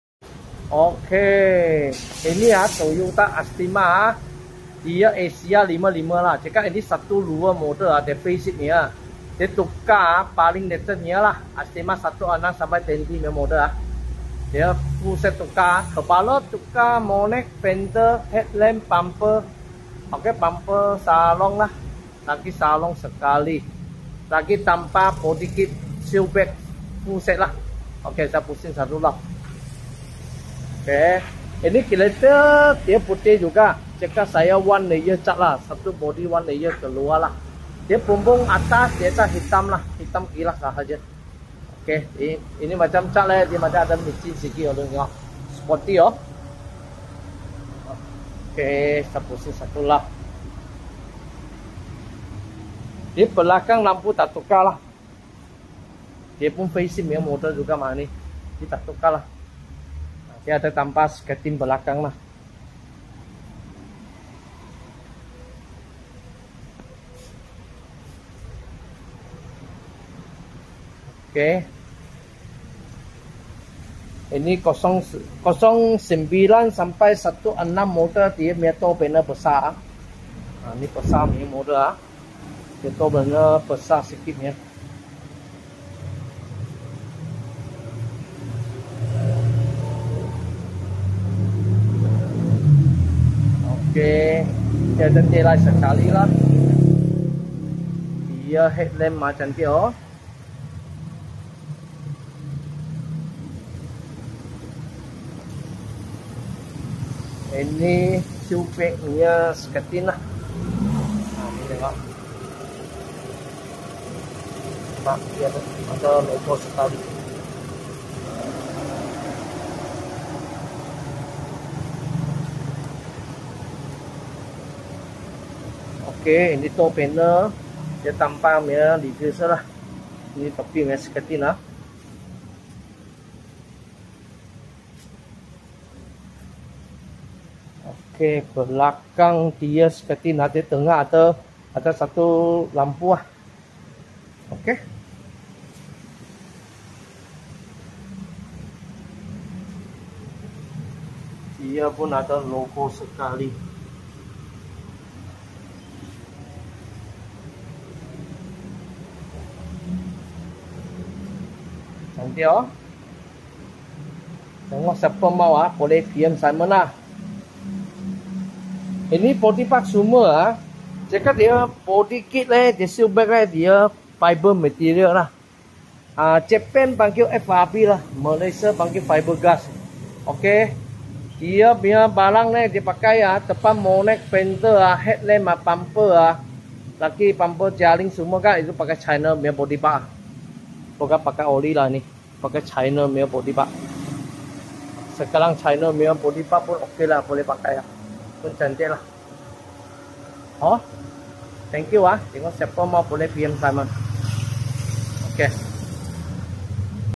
TOYOTA ESTIMA ACR50/55 2006-15 CONVERT TO FACELIFT 2016-20 BODY PARTS (J-EMOTION DESIGN)
TOYOTA ESTIMA 2006-15 ACR50/55 CONVERT TO ESTIMA FACELIFT 2016-20 BODY PARTS AND BODY KIT

1. 2016 FRONT HEAD LAMP 1SET
2. 2016 FRONT BUMPER FULL SET
3. 2016 FRONT GRILLE 1PCS
4. 2016 FRONT FENDER 1SET
5. 2016 FRONT BONNET 1PCS
6. BODY PARTS MADE IN CHINA 🇨🇳🇨🇳🇨🇳
- SILK*BLAZESTYLE FRONT LIPS 1PCS
- SILK*BLAZESTYLE SIDE SKIRT 4PCS
- SILK*BLAZESTYLE REAR SKIRT 1PCS
- LED REAR REFLECTOR 1SET

PLS CALL AND WHATSAPP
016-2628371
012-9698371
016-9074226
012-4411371